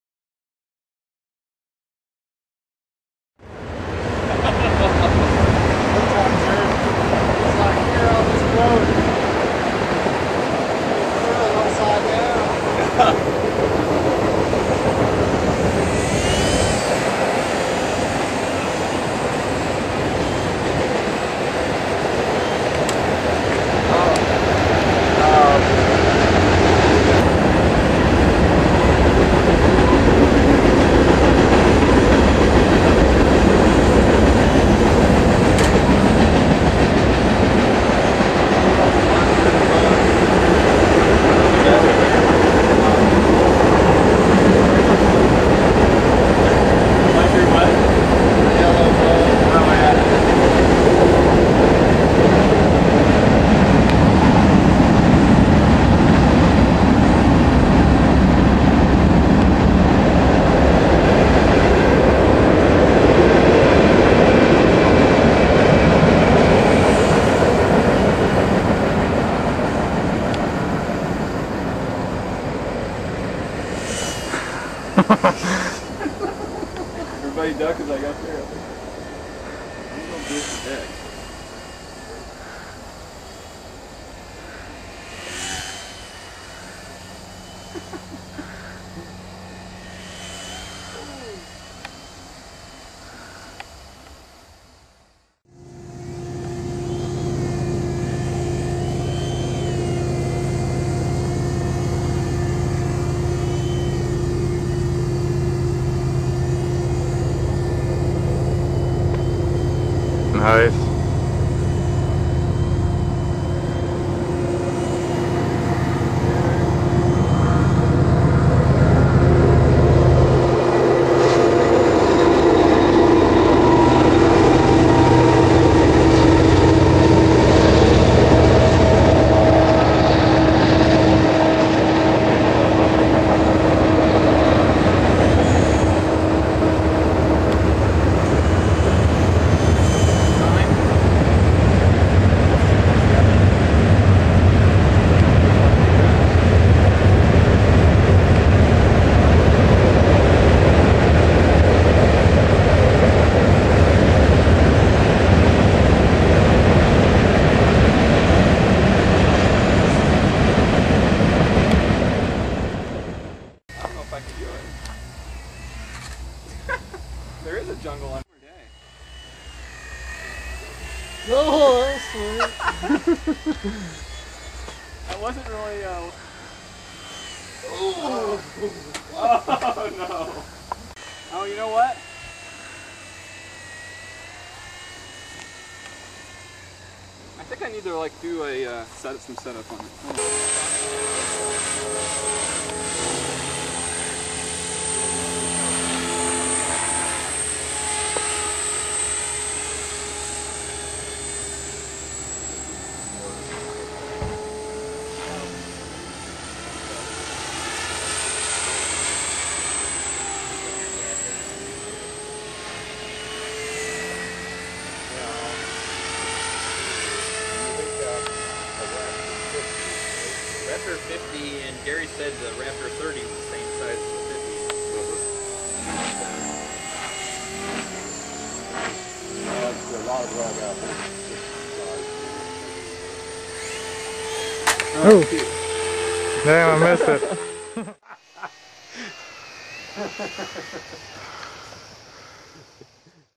Blade mSR at the field
Jared Granzow flying the E-Flite Blade mSR at C.O.R.F. flying field